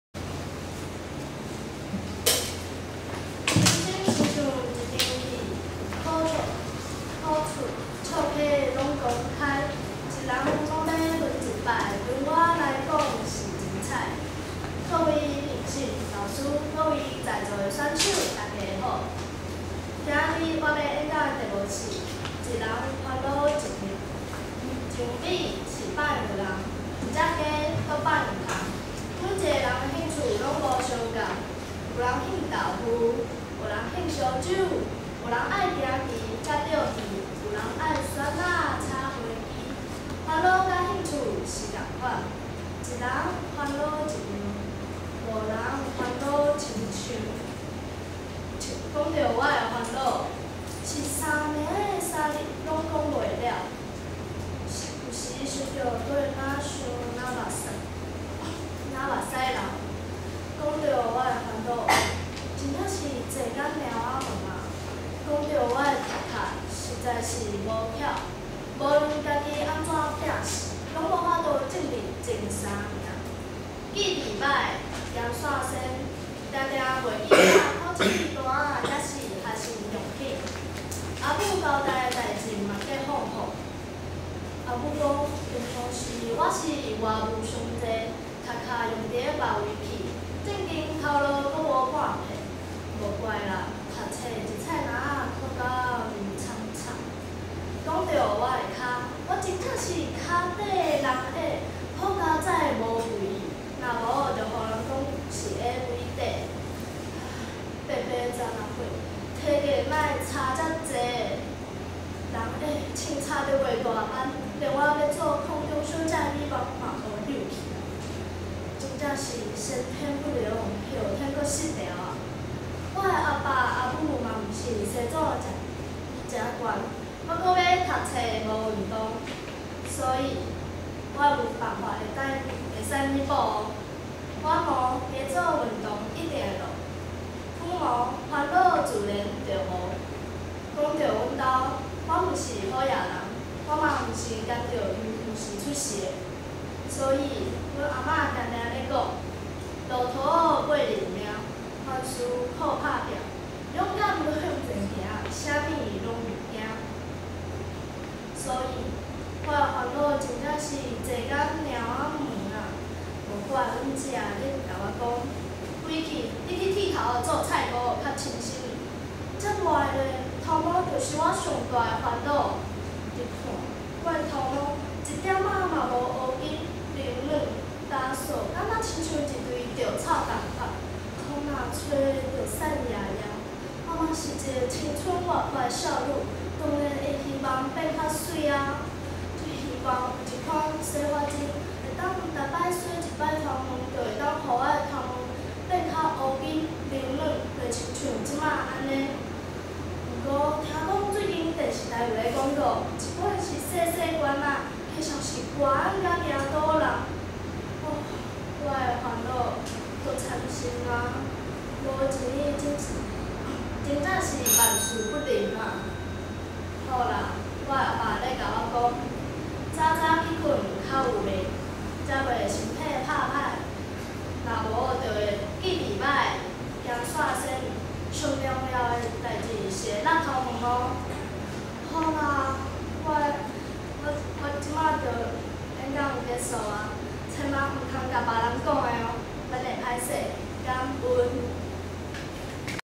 台語演講比賽-國中組入決賽_永吉國中(一人煩惱一樣)陳姵君
Upload by Jimmy.